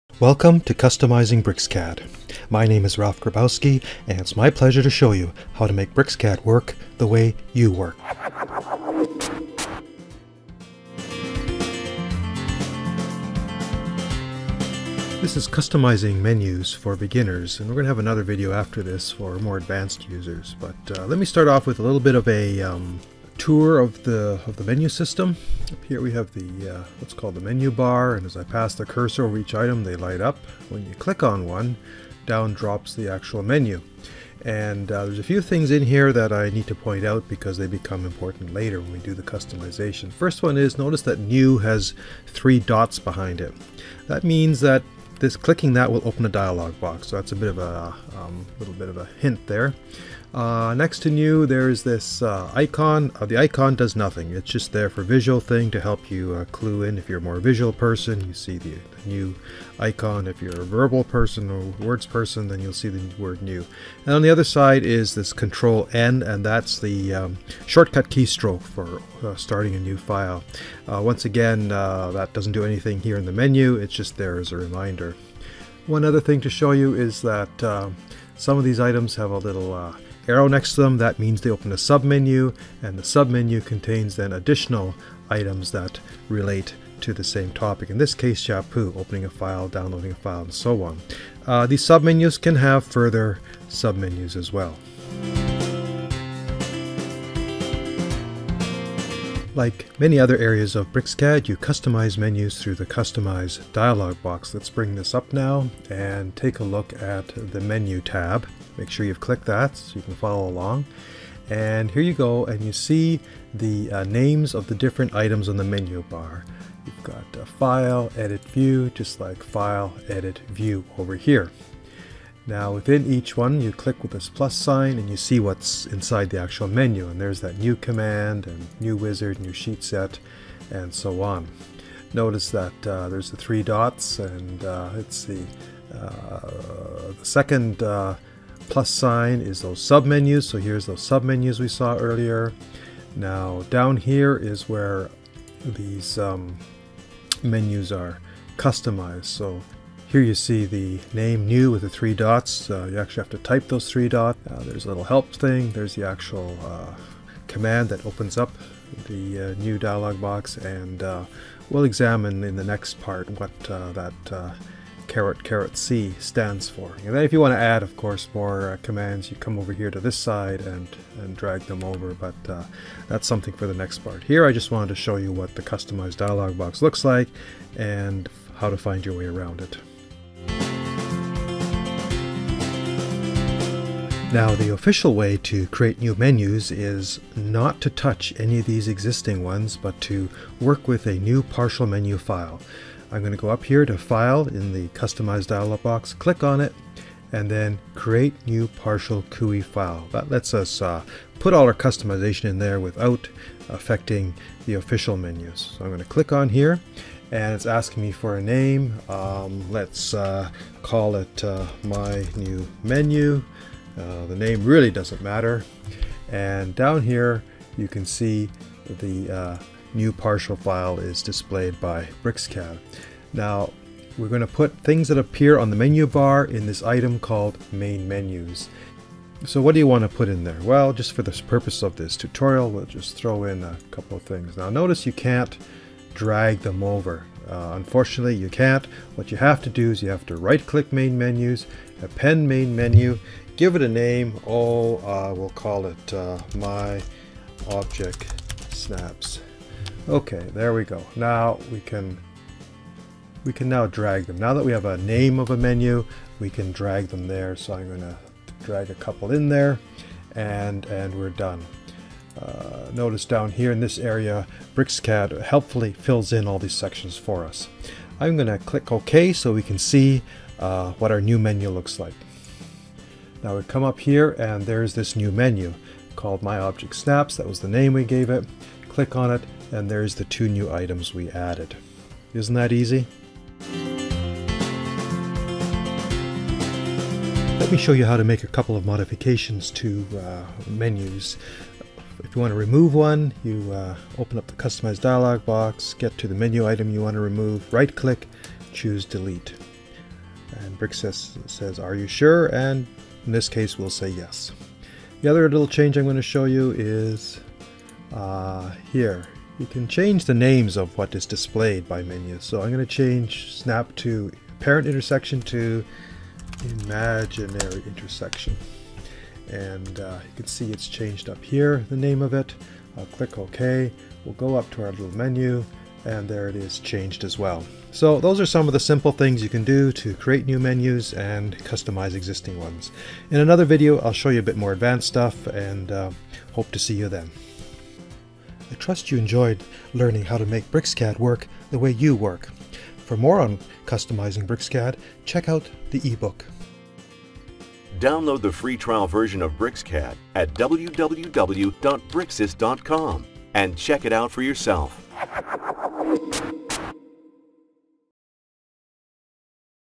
Easy Menu Customization
The menu bar and menus can be completely customized in BrisCAD. In this video, you learn how to make a new menu to the bar, add commands to the menu, and then to rename and remove menu items.